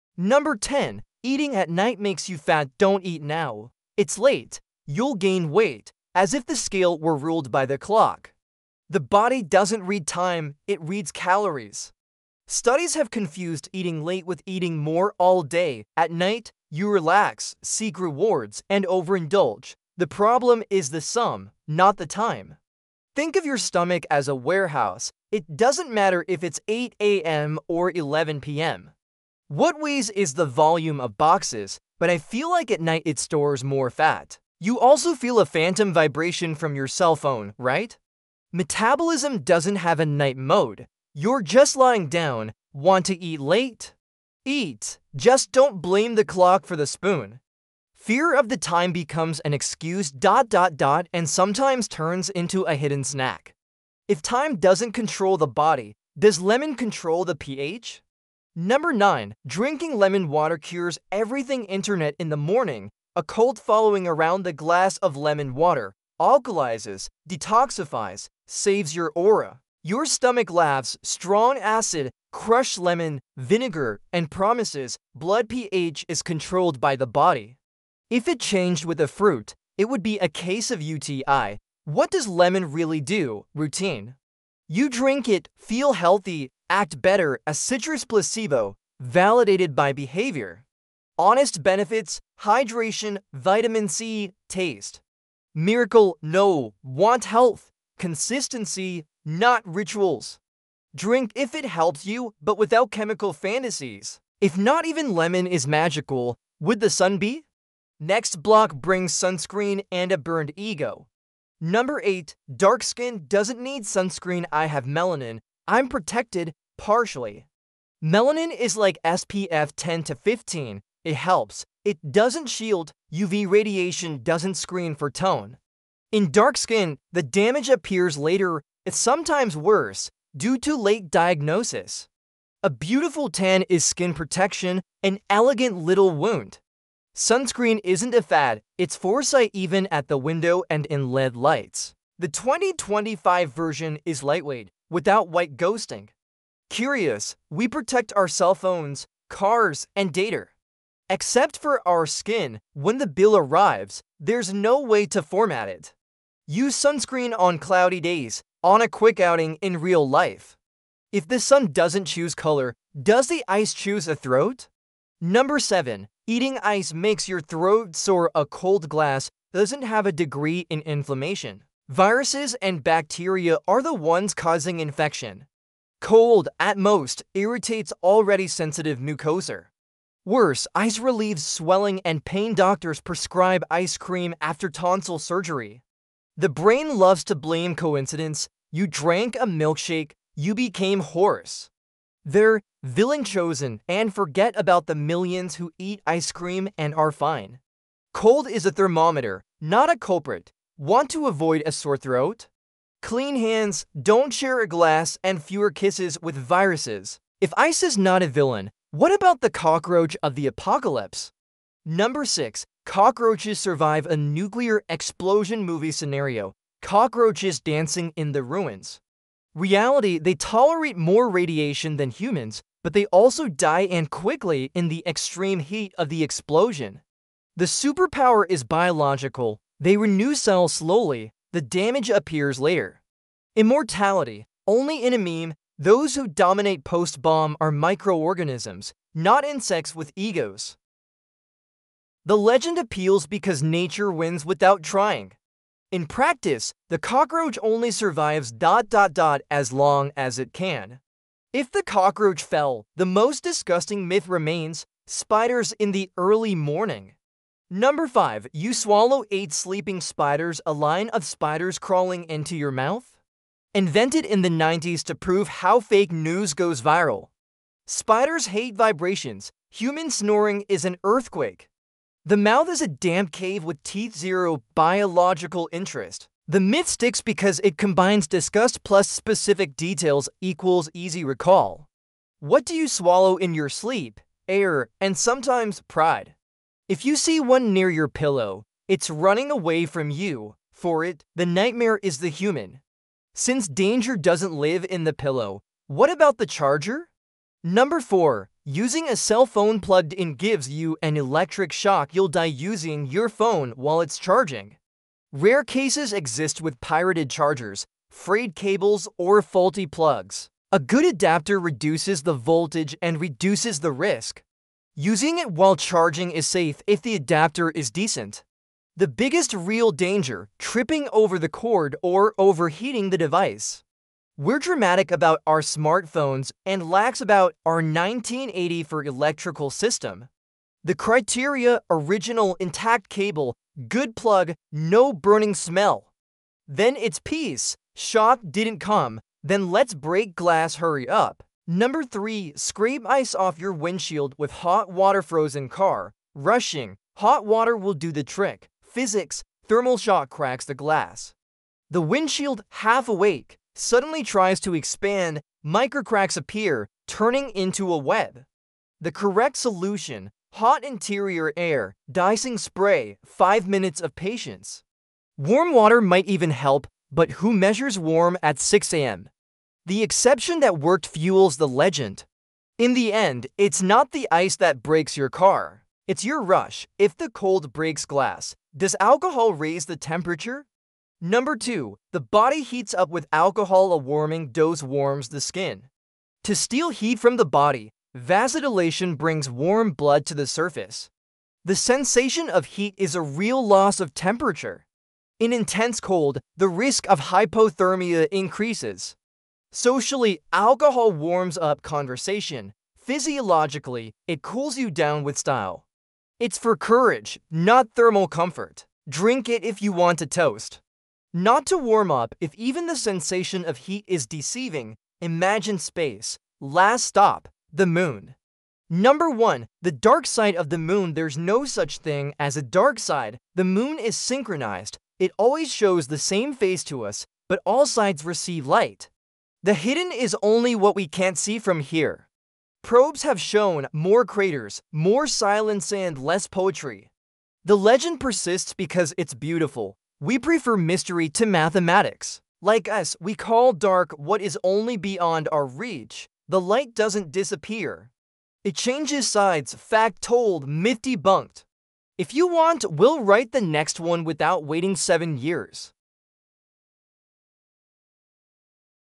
10 Truths That Only Existed in Our Heads — Explained
You believed it all — until now.
In this video, we reveal 10 truths that only existed in our heads: popular myths, false beliefs, and “scientific facts” that time and science have already proven wrong.
Get ready to discover what really happens with lemon water, nighttime metabolism, sunscreen, the apocalypse cockroaches, the alcohol that ‘warms you up’, and even the so-called dark side of the Moon.

🔍 No guesses here — just facts, history, and science told in a way you’ve never seen before.
If you love scientific curiosities, real mysteries, human behavior, and hidden truths, this video is for you.